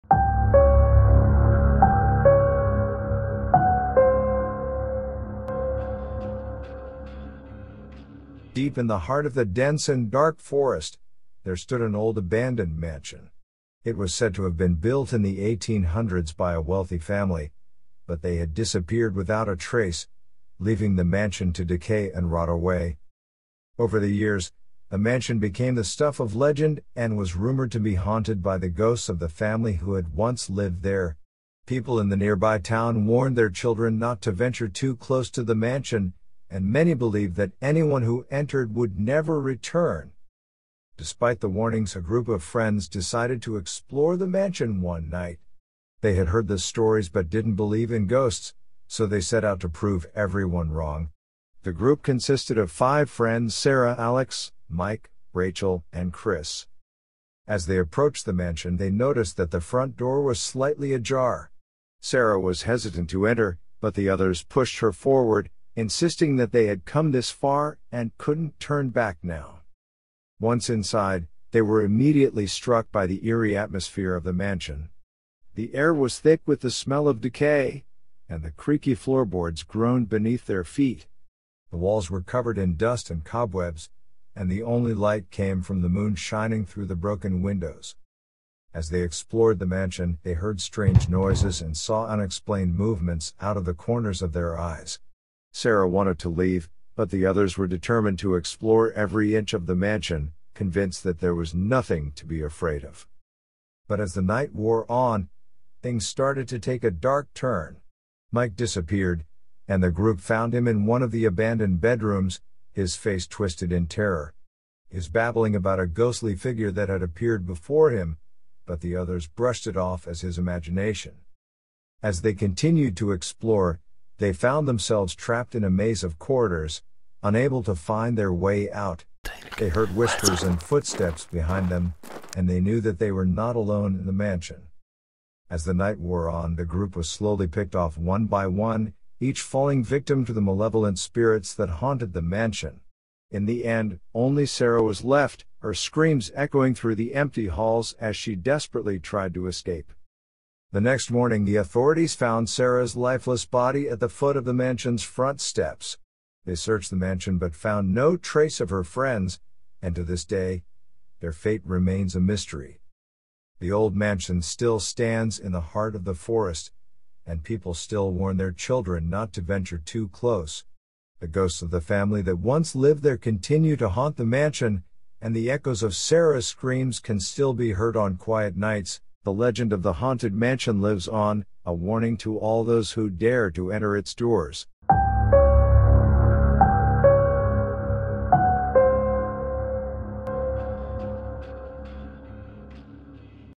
Exploring a Spooky Abandoned Mansion
Are you looking for an exciting and thrilling adventure? Then join us as we explore an abandoned mansion with its dark corners and hidden secrets. From mysterious music to creaky doors, this creepy exploration will surely have you on the edge of your seat. So come along if you dare and be prepared for the surprises this abandoned mansion has in store.